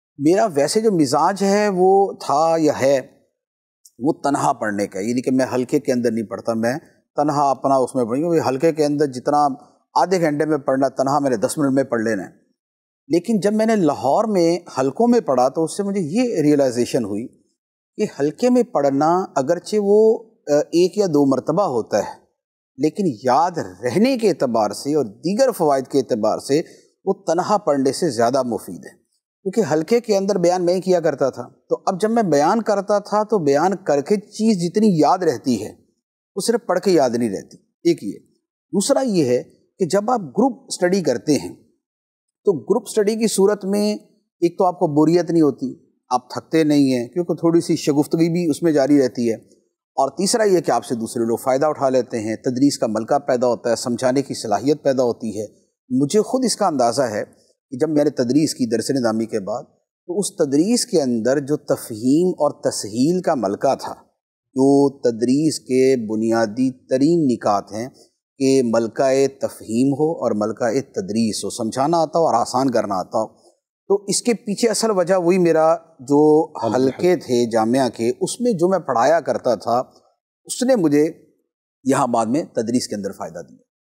Group Study Vs Solo Study | Group Study Kay Fawaid | Mufti Muhammad Qasim Attari Ka Tajriba
Topic: Group Study Vs Solo Study | Group Study Kay Fawaid | Mufti Muhammad Qasim Attari Ka Tajriba
Group Study Vs Solo Study
مفتی قاسم صاحب کی نظر میں بہترین کیا؟